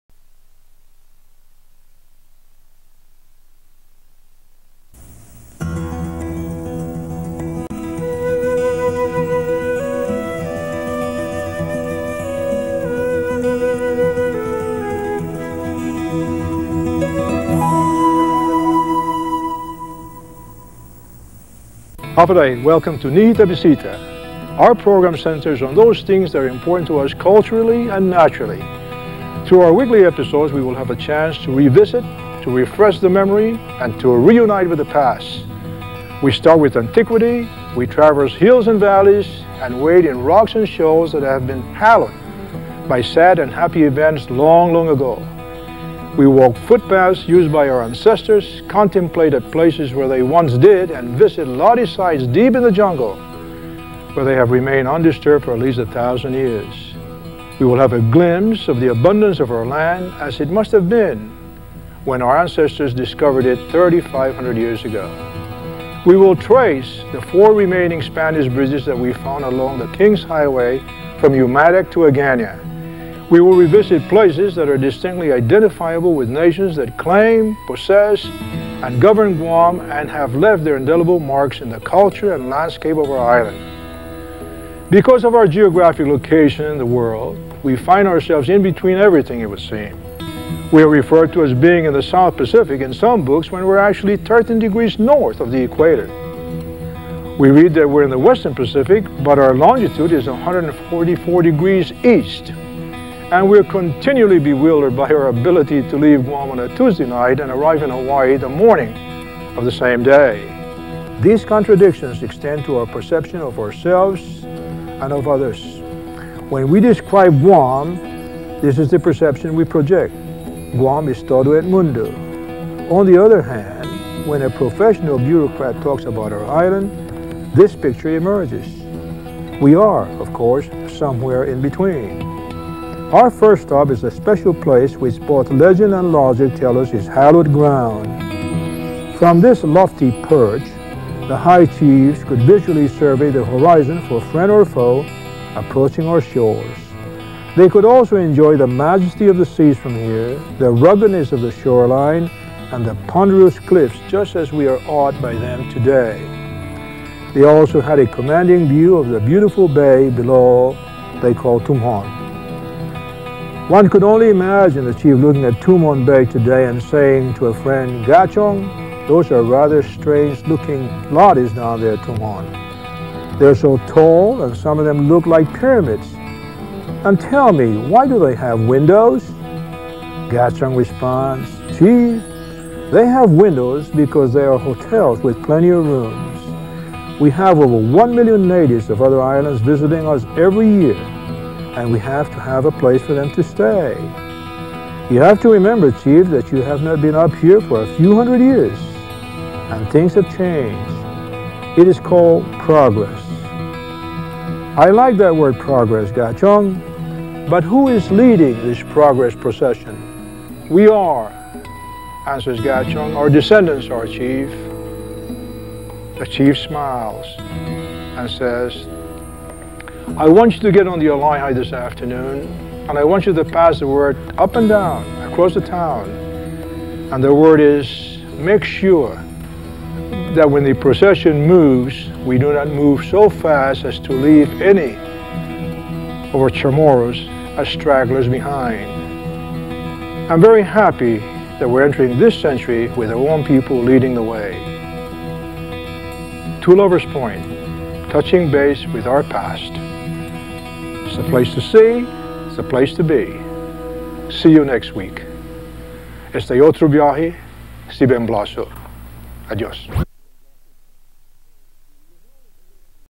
BisitaGuam E01: Two Lover's Point
-- Guam's native son, former Brig. General and US Congressman Ben Blaz reflects on events and circumstances that have markedly affected his people.